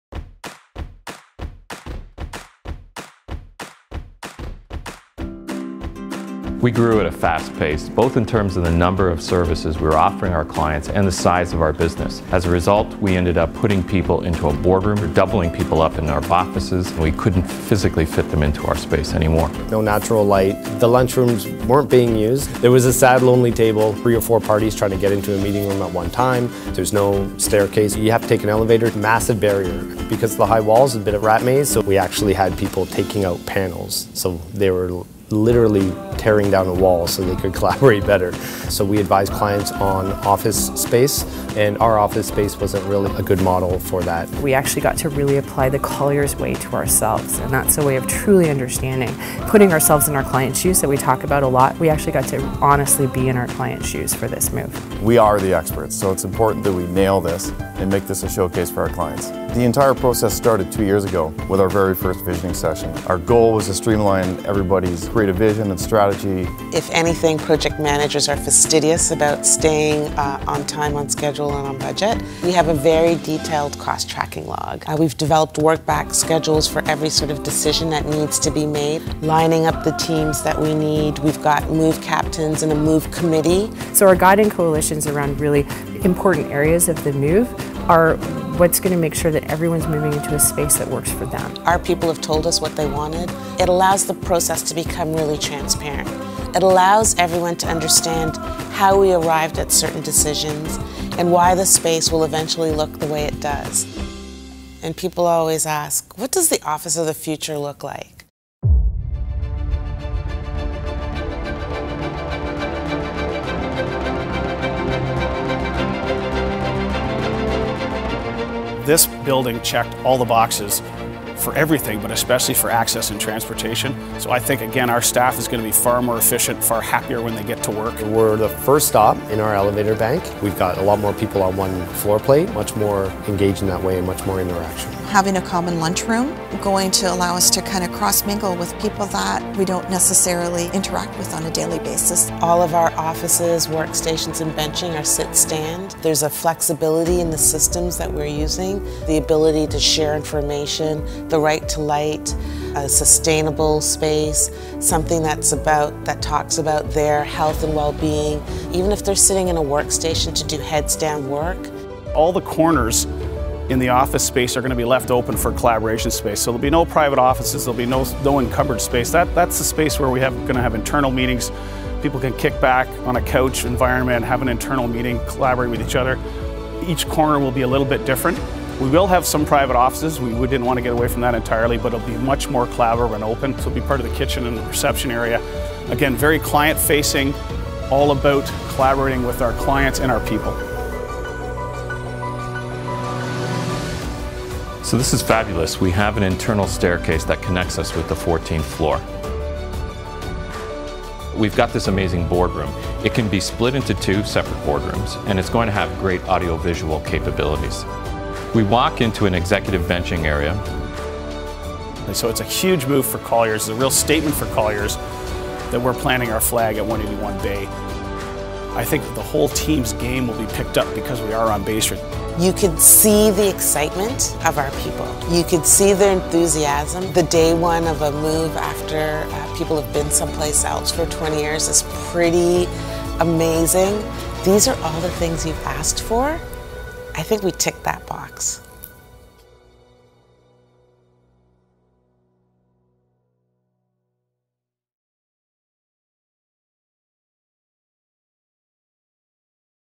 Colliers International Downtown Toronto Move
Colliers International decided it was time to move and modernize its Toronto Downtown office, choosing Brookfield Place, at 181 Bay St, as its new home.

Colliers transformed its workplace culture into one fostering greater collaboration, transparency and innovation, demonstrating the company’s values of service, expertise, community and fun. The office at 181 Bay Street provides areas for focused thought, shared work, continuous learning and consistent interaction, moving away from a purely functional space to an agile, flexible environment. Integrating the same approach, it takes to solve client’s business and space challenges, Colliers was able to provide employees with a workspace where they can thrive and succeed.

Daniel Holmes
Senior Managing Director | Broker
Office Practice Group, Toronto Region